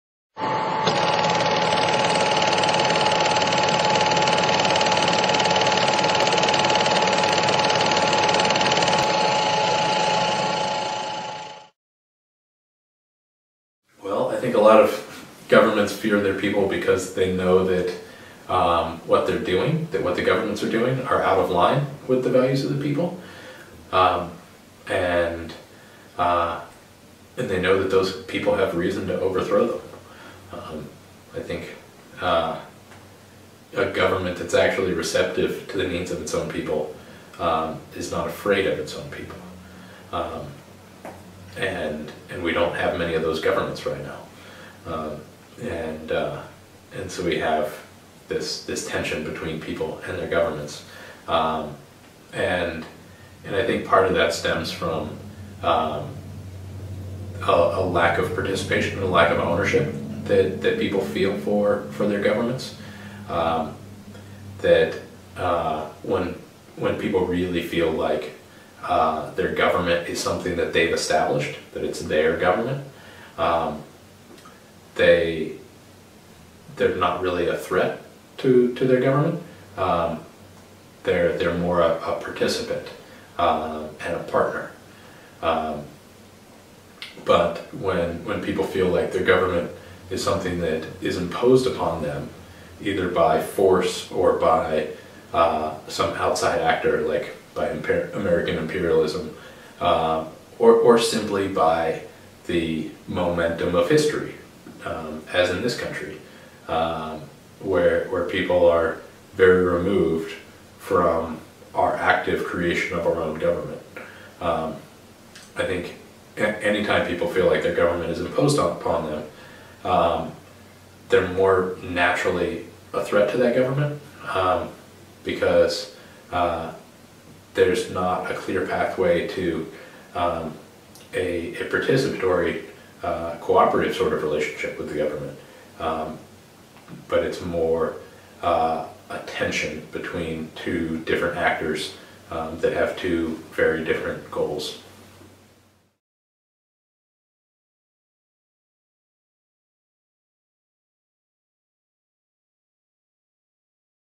Tim DeChristopher - Why do so many governments fear their own people?
Tim DeChristopher, climate activist and co-founder of the group Peaceful Uprising, spoke with the Weave in Canton, NY in September 2013.